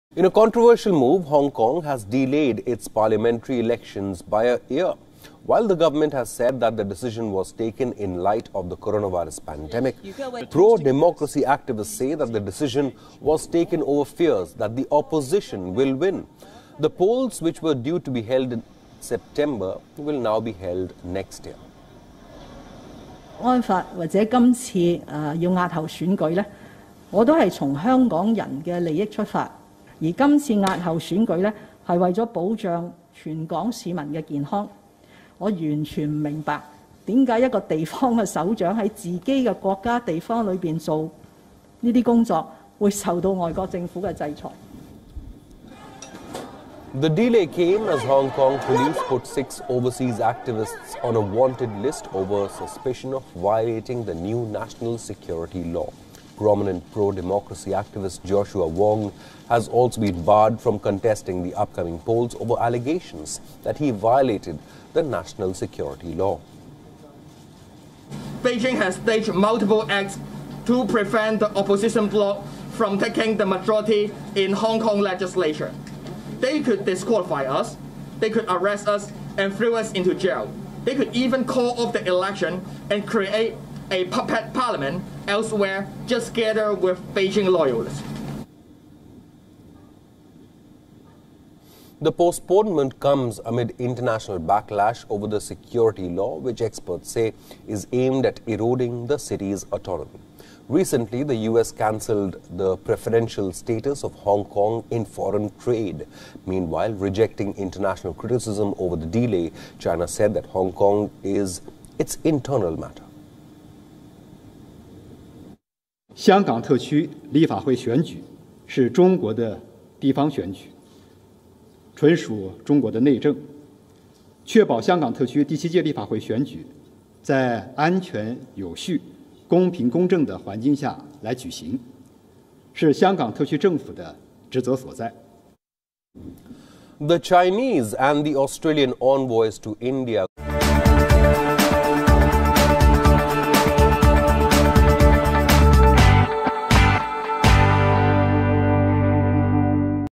Hong Kong delays its Parliamentary polls over pandemic, opposition protests World News
Hong Kong delays its Parliamentary polls over pandemic. Opposition protests over the decision of the present government.